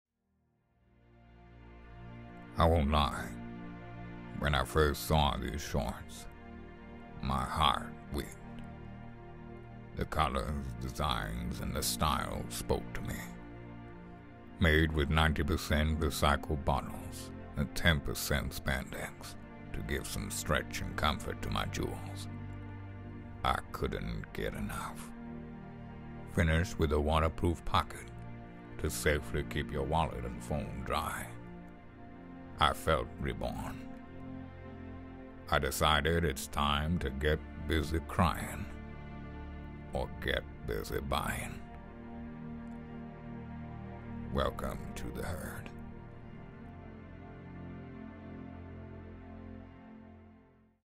Randy Cow Swim Shorts
Stunning swim shorts with a waterproof pocket.  Made from 90% recycled plastic bottles and 10% Spandex for the ultimate fit and comfort.